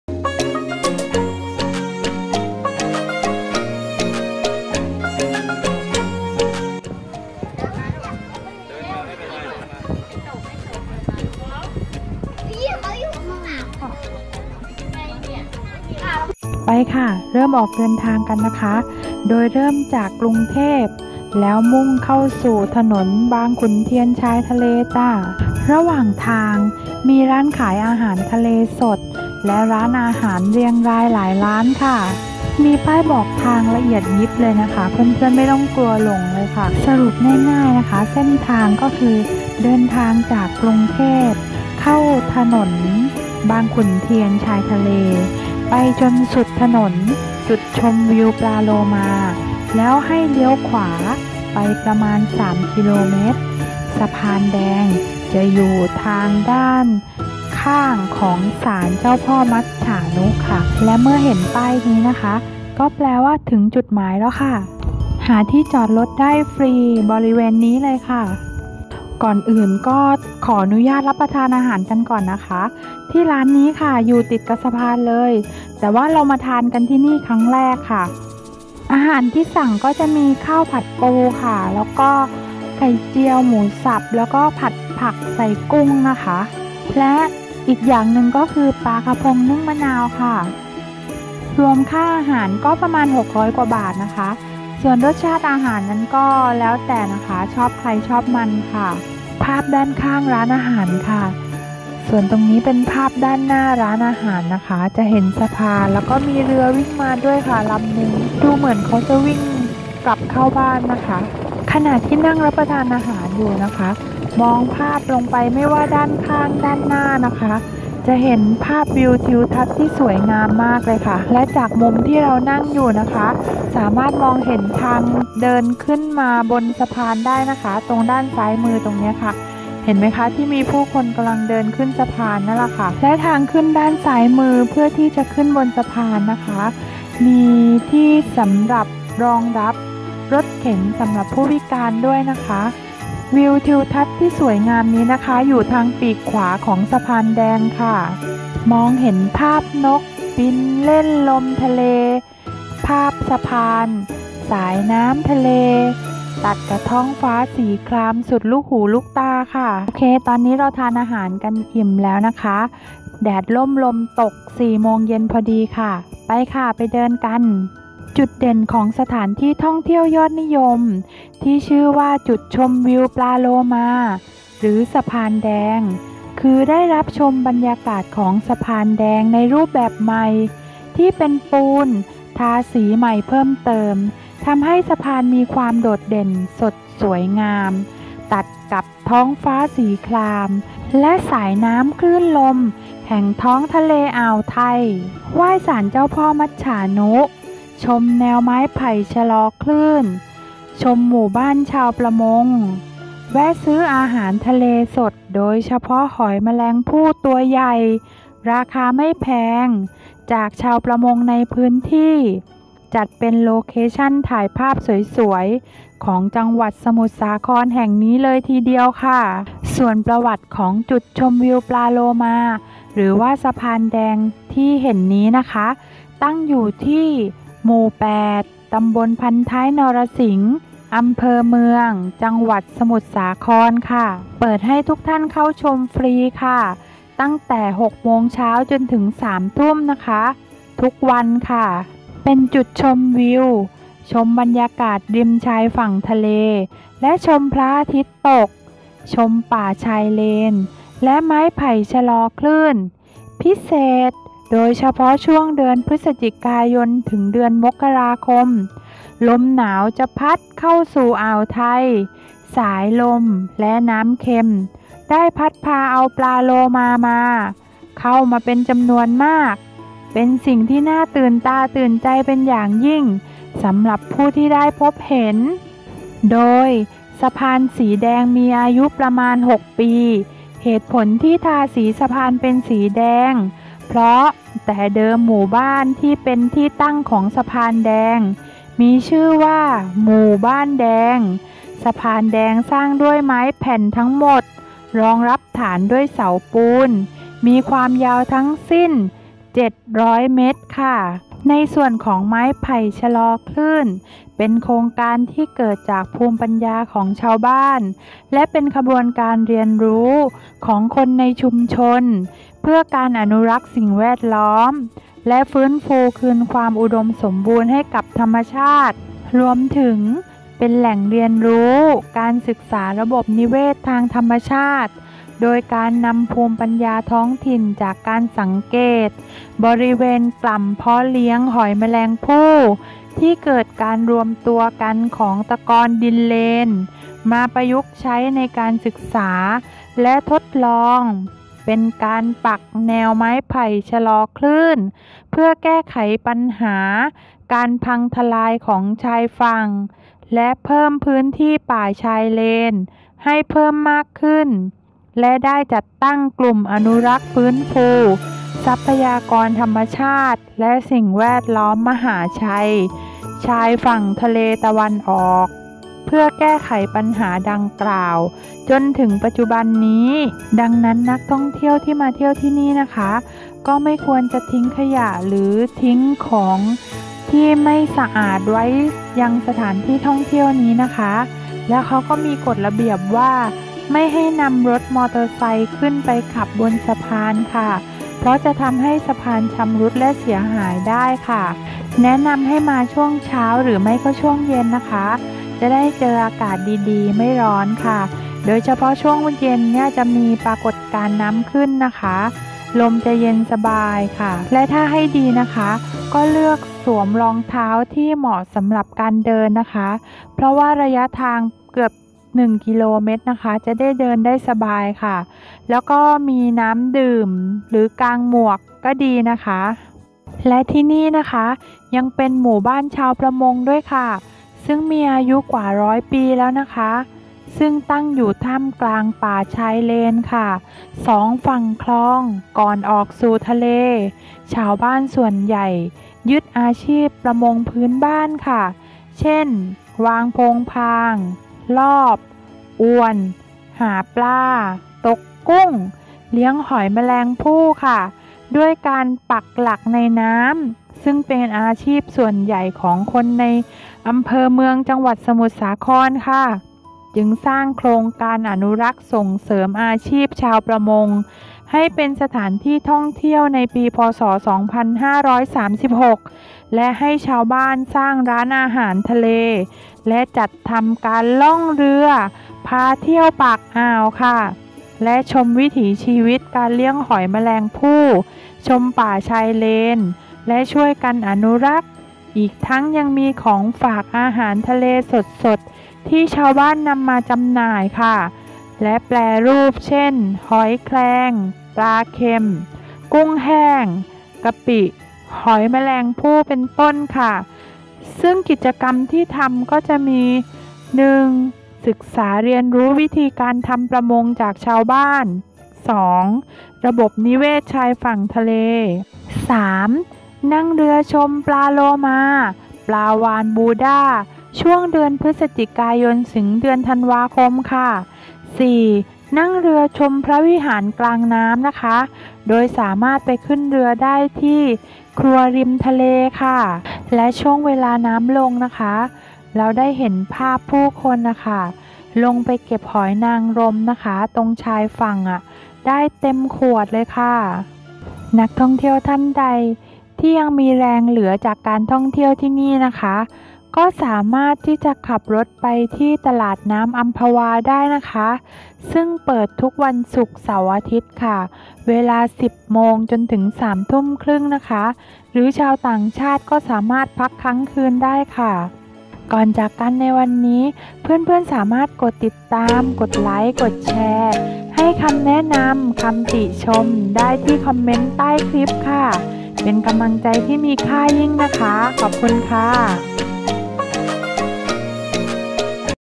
สะพานแดง วิวสวยสุดว้าว! แลนด์มาร์กชมโลมาใกล้กรุงเทพ แถมร้านอาหารเพียบ (ล่าสุด)
สะพานแดง จุดชมวิวปลาโลมา  ตั้งอยู่ที่ตำบลพันท้ายนรสิงห์ อำเภอเมือง จังหวัดสมุทรสาคร เป็นสะพานไม้สีแดงที่ทอดยาวขนานไปกับชายทะเลความยาว 700  เมตร  มองเห็นวิวทิวทัศน์ของชายฝั่งที่สวยงาม มีลมทะเลพัดเย็นสบาย  สามารถชมบรรยากาศของพระอาทิตย์ขึ้นและตก ชมป่าชายเลนและไม้ไผ่ชะลอคลื่น  ปัจจุบันมีการปรับปรุงทางเดินบริเวณสะพานไม้แดงใหม่เป็นแบบพื้นปูนซึ่งมั่นคงขึ้น
แต่ก่อนในช่วงเดือน พฤศจิกายน-มกราคม ลมหนาวเข้าสู่อ่าวไทย สายลมและน้ำเค็มได้พัดพาเอาฝูงปลาโลมาเข้ามาเป็นจำนวนมาก จุดนี้จึงเป็นจุดชมปลาโลมาจนเป็นที่มาของชื่อ แต่ปัจจุบันโอกาสที่จะได้เห็นปลาโลมามีน้อยลงหรืออาจจะไม่เห็นเลย แต่จุดชมวิวก็ยังเป็นที่นิยมของนักท่องเที่ยว บริเวณทางเข้ามีลานจอดรถกว้าง และร้านอาหารทะเลให้บริการหลายร้าน ทานอาหารแล้วมาเดินเล่นชมวิวที่สะพานแดงต่อ หากมาช่วงกลางวันแดดจะค่อนข้างแรงมาก แนะนำให้พกร่ม หมวก มาให้พร้อม
เดินเล่นชมวิวถ่ายภาพตามมุมต่างๆ ของสะพานไปเรื่อยๆ มองไปเบื้องหน้า คือ วิวทิวทัศน์ของชายทะเลกว้างไกลสุดตา ถึงแม้แดดจะแรง แต่มีลมพัดเข้ามาตลอดช่วยคลายร้อนไปได้บ้าง
ช่วงสุดปลายสะพาน มีศาลาชมวิวให้นั่งพักผ่อน
การเดินทาง : จากกรุงเทพฯ ให้เข้าถนนบางขุนเทียน-ชายทะเล ให้ไปจนสุดถนนจุดชมปลาโลมาแล้วให้เลี้ยวขวา ไปประมาณ 3 กิโลเมตร สะพานแดงจะอยู่ด้านข้างของศาลเจ้าพ่อมัจฉาณุ